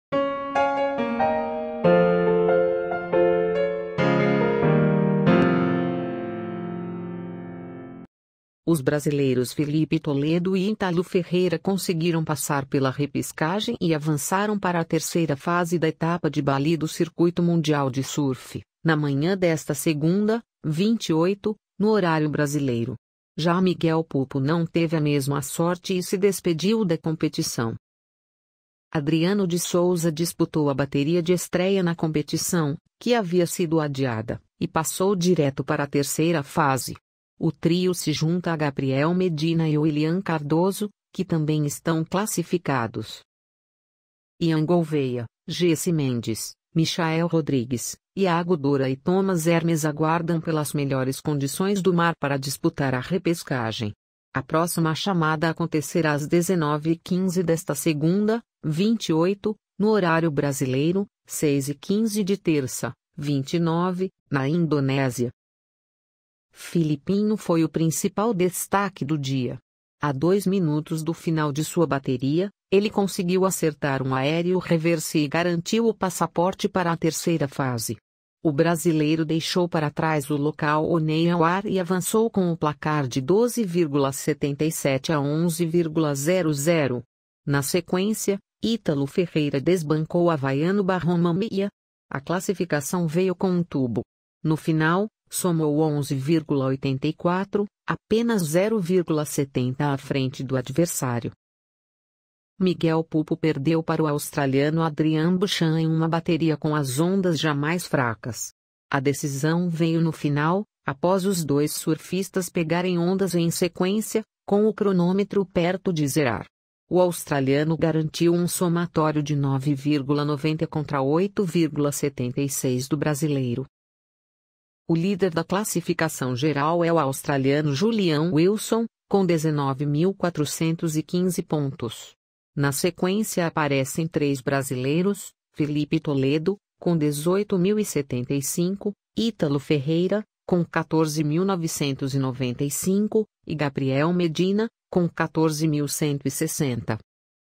Filipinho bate surfista local e avança em Bali
Os brasileiros Filipe Toledo e Ítalo Ferreira conseguiram passar pela repescagem e avançaram para a terceira fase da etapa de Bali do Circuito Mundial de Surfe, na manhã desta segunda (28), no horário brasileiro. Já Miguel Pupo não teve a mesma sorte e se despediu da competição.